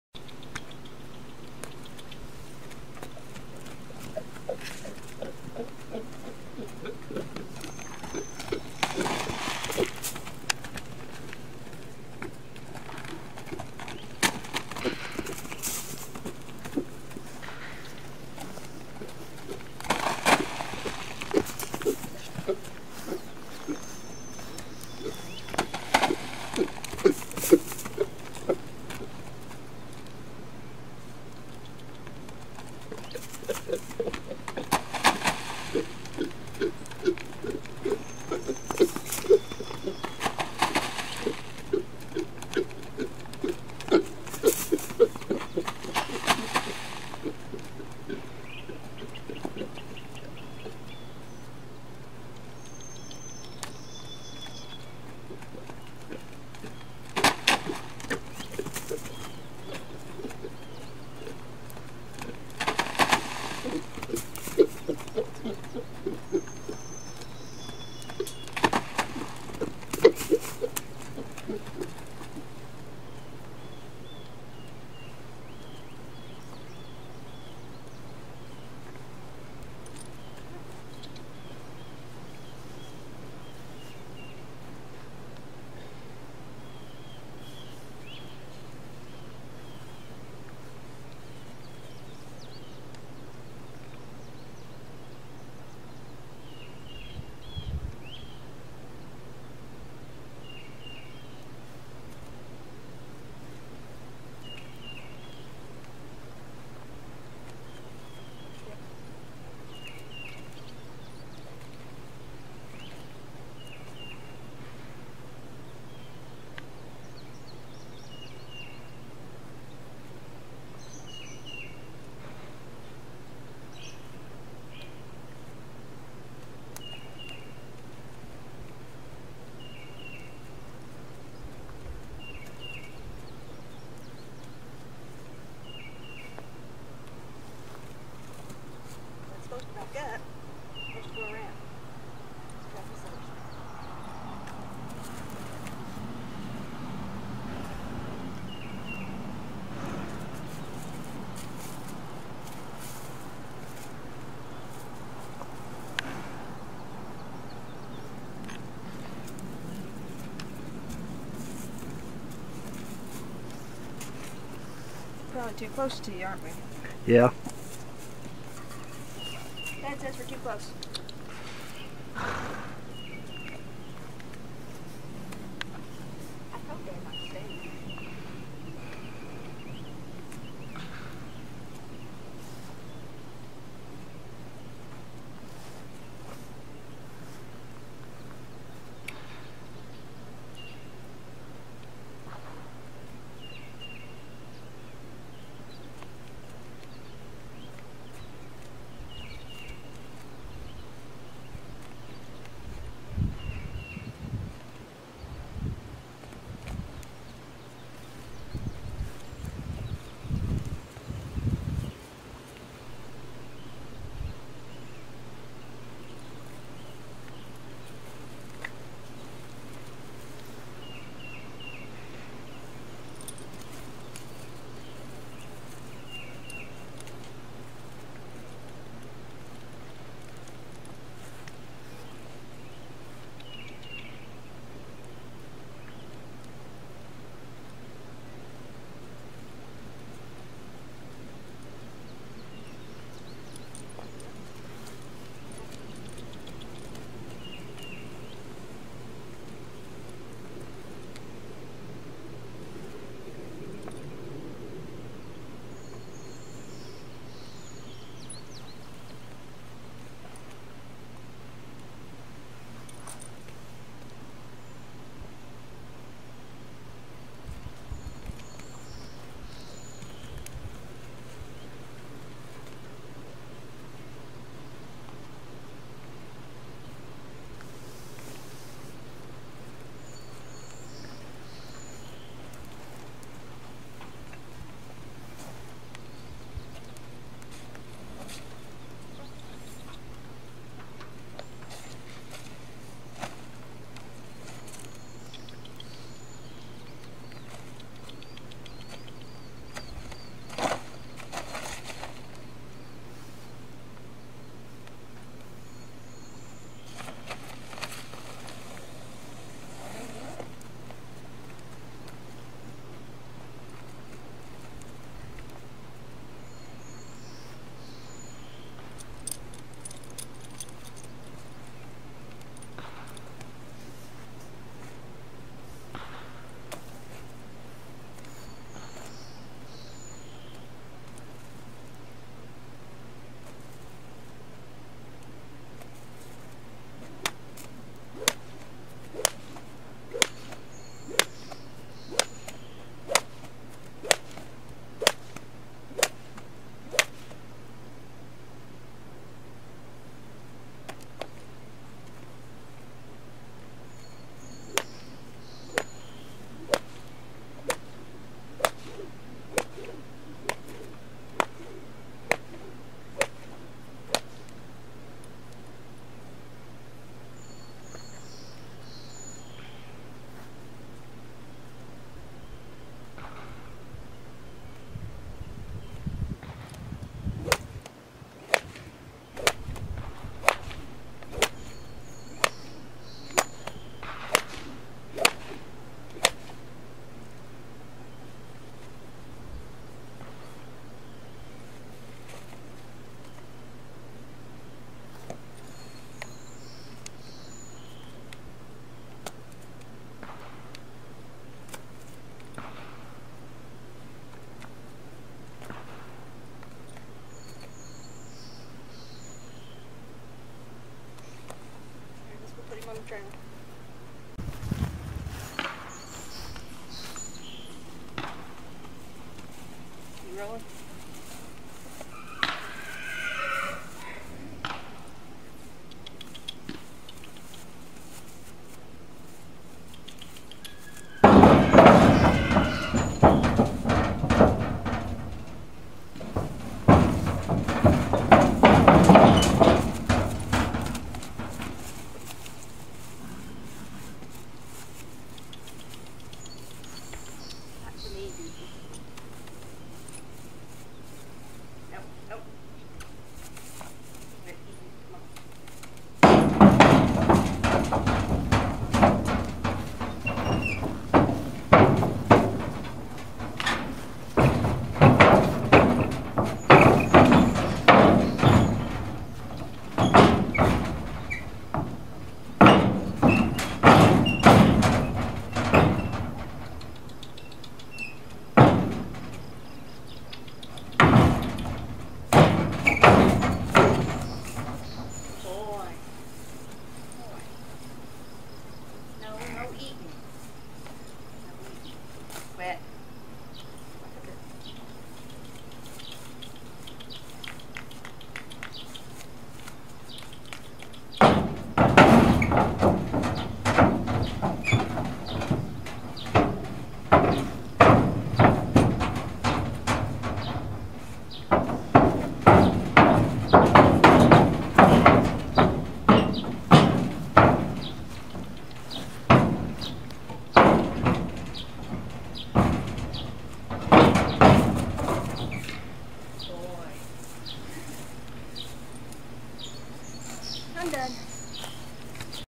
Red Zeppelin ground work & trailer loading on 6-4-14
Wonderful Rocky yearling gelding - learns sooo quickly!  Vet recommended a few minutes of lunging every day this week to help keep swelling down from his gelding - so why not film it and add a few more exercises - he's doing great and he's beautiful! Love this guy!